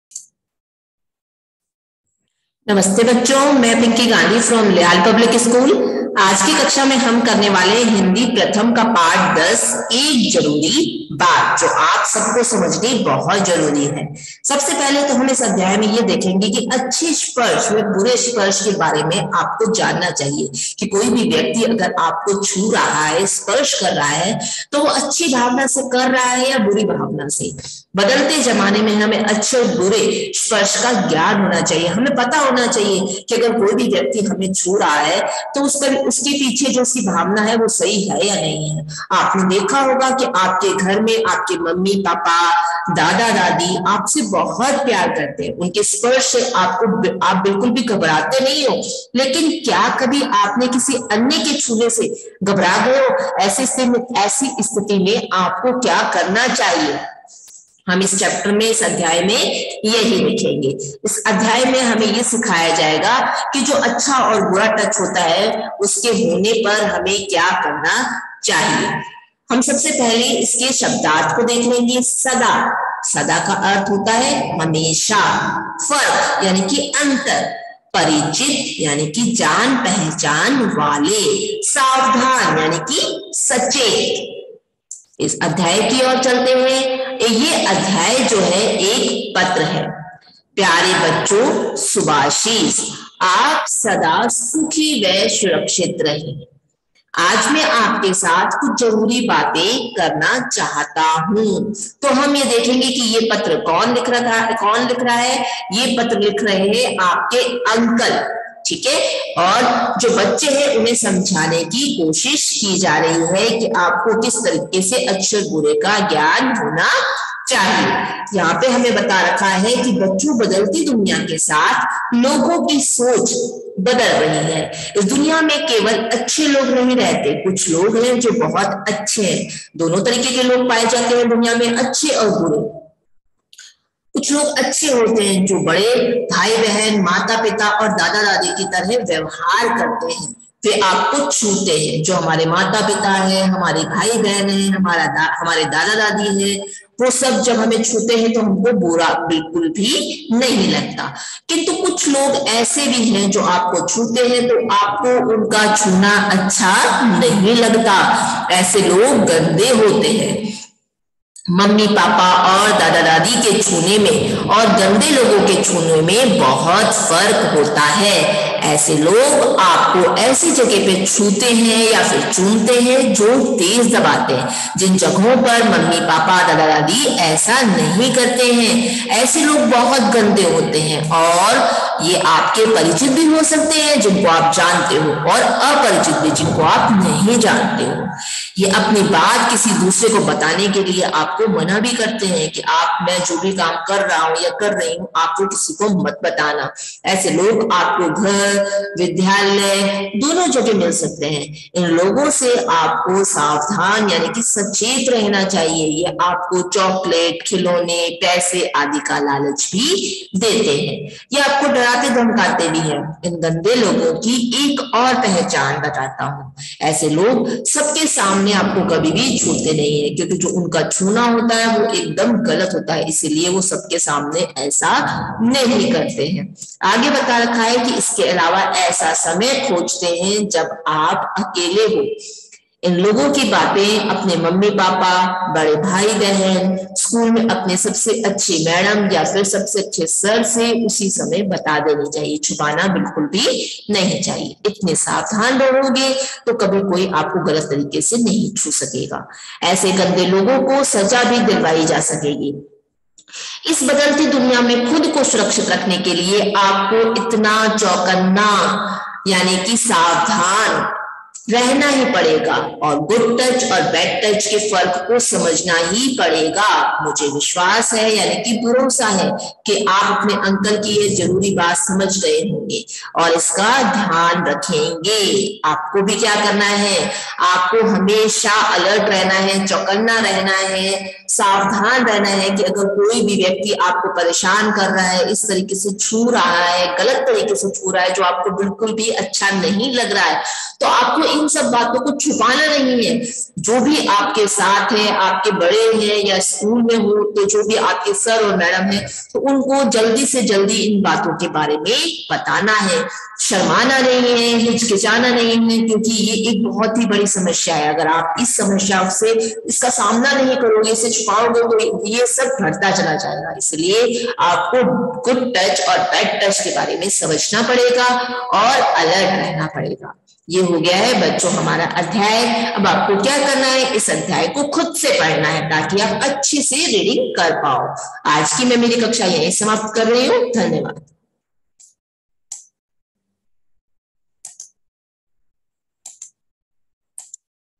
Class - III - Hindi - Online Class 10-01-2022 by Pinky Ma'am (LPS)
एक जरूरी बात ( पाठ १० )